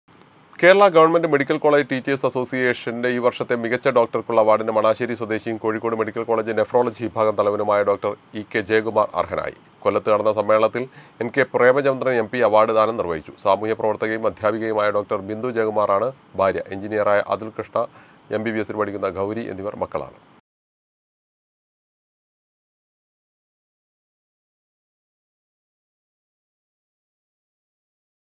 കേരള ഗവൺമെൻറ് മെഡിക്കൽ കോളേജ് ടീച്ചേഴ്സ് അസോസിയേഷൻ്റെ ഈ വർഷത്തെ മികച്ച ഡോക്ടർക്കുള്ള അവാർഡിന്
കേരള ഗവൺമെൻറ് മെഡിക്കൽ കോളേജ് ടീച്ചേഴ്സ് അസോസിയേഷൻ്റെ ഈ വർഷത്തെ മികച്ച ഡോക്ടർക്കുള്ള അവാർഡിന് മണാശ്ശേരി സ്വദേശിയും കോഴിക്കോട് മെഡിക്കൽ കോളേജ് നെഫ്രോളജി വിഭാഗം തലവനുമായ ഡോക്ടർ ഇ'കെ ജയകുമാർഅർഹനായി